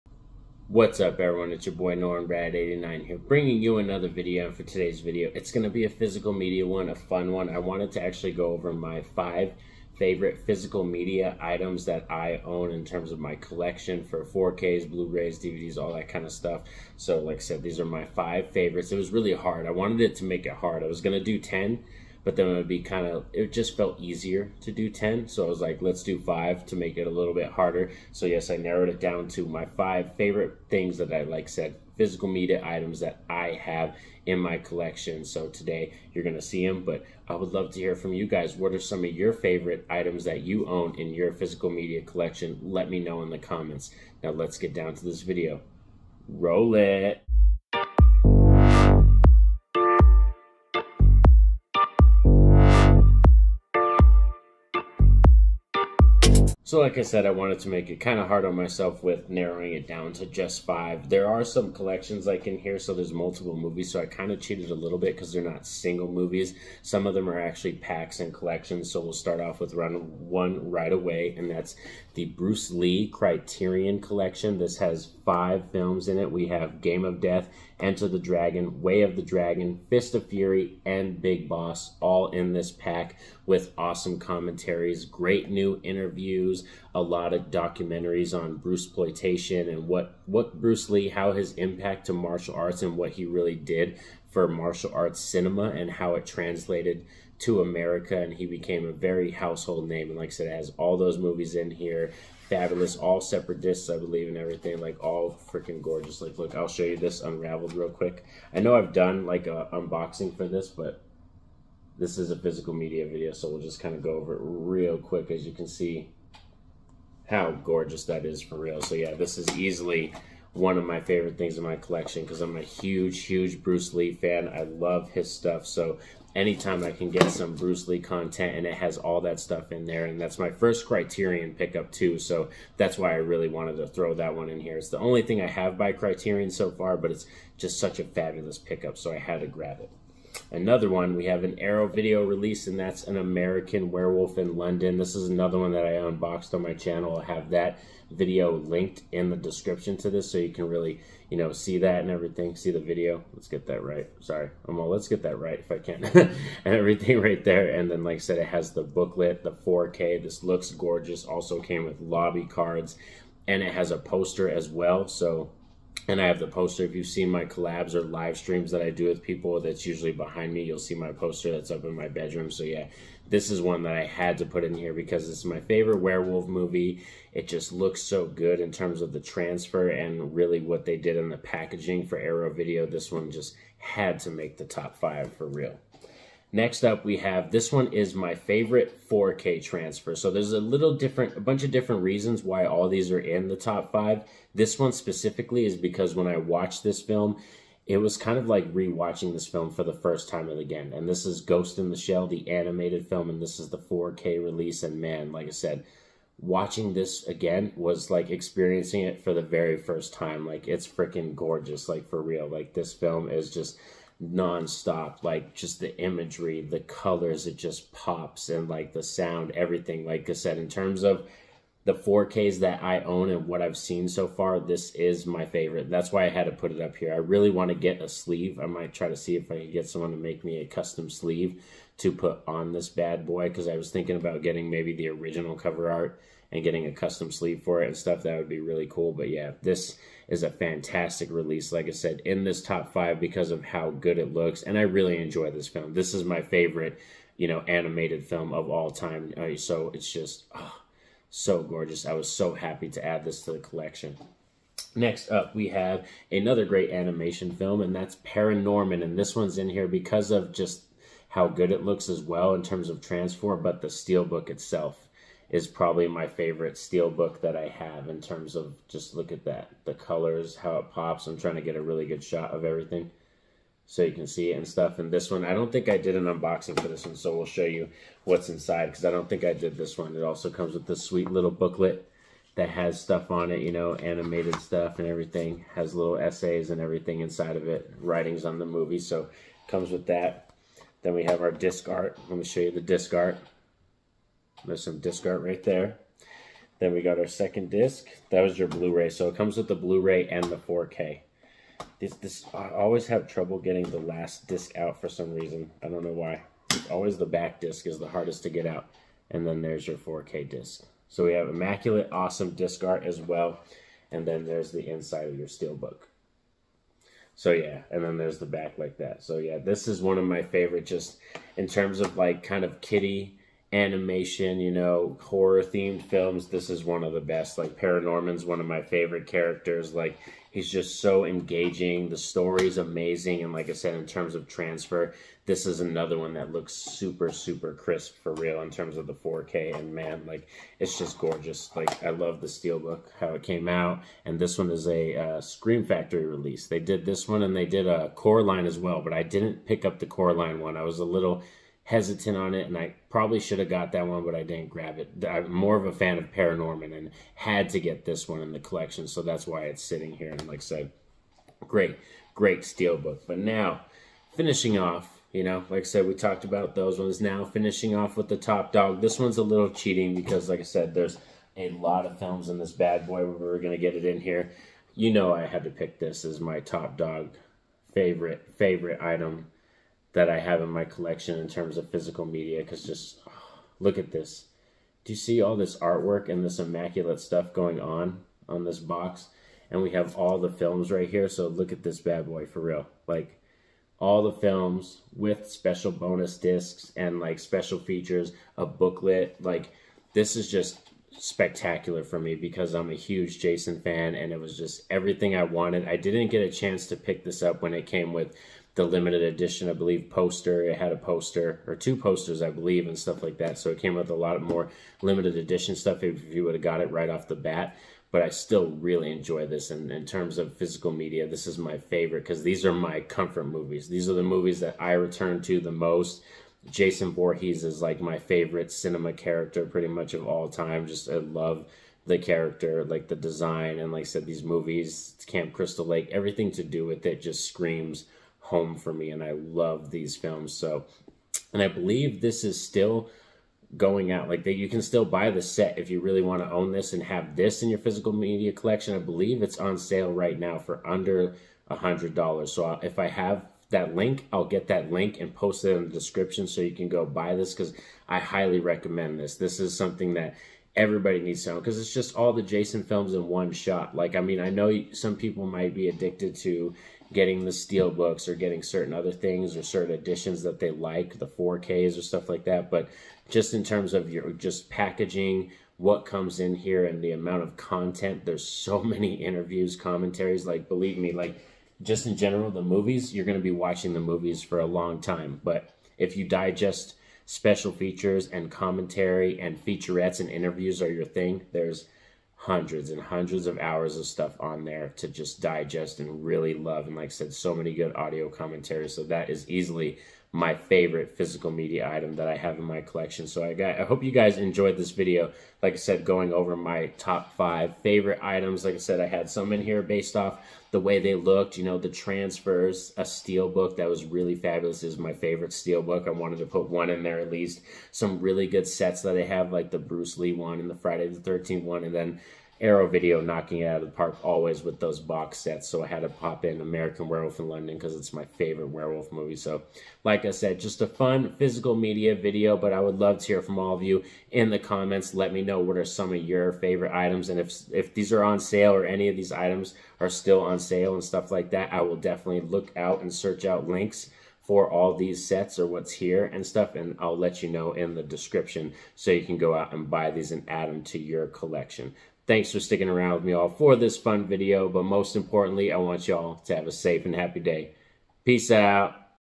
Top 5 favorite Physical Media
Today I wanted to go over my Five favorite physical media items I own.